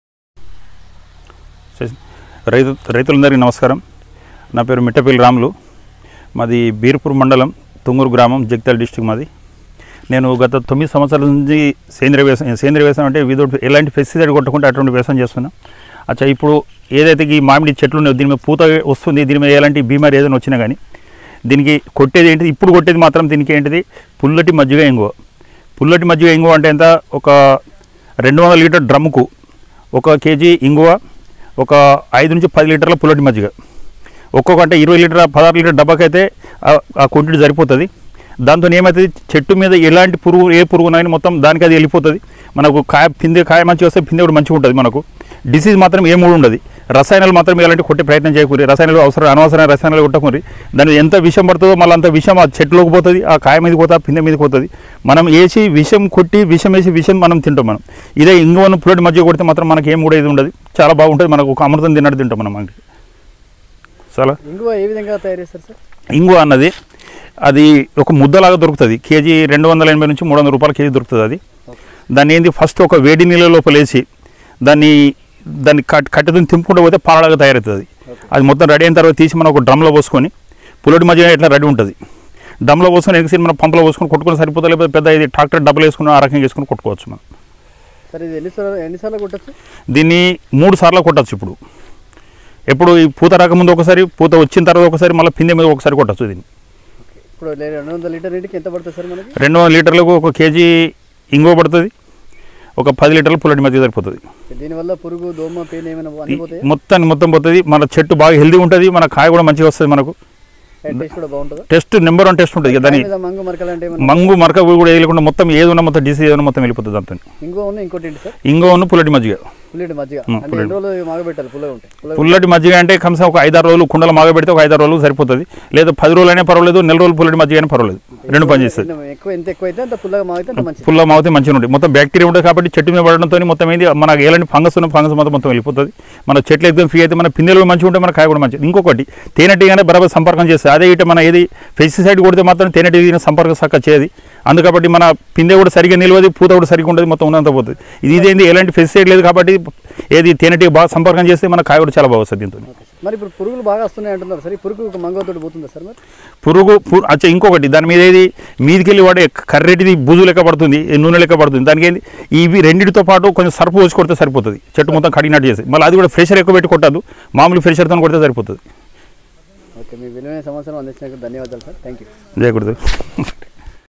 సేంద్రీయ పద్దతిలో మామిడి సాగు చేస్తున్న మంచి ఆదాయం వస్తుంది రైతు మిట్టపెల్లి రాములు/భరత్ రైతు బడి👨‍🌾
mittapelly ramulu#bharath raithubadi#